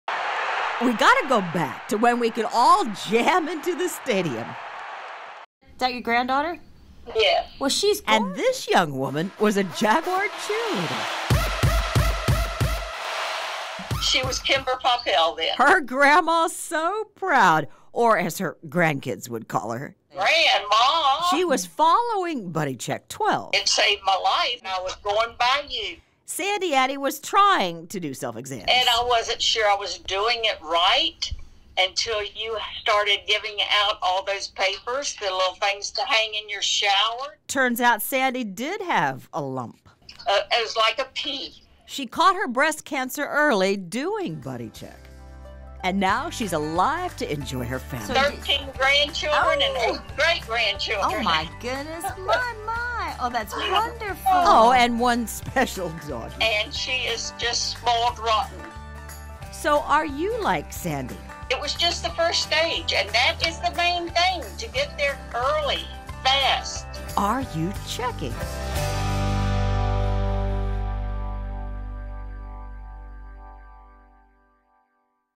'It felt like a little pea:' Jacksonville grandmother catches her breast cancer early
Sandy Addy did a self-exam and it saved her life through Buddy Check.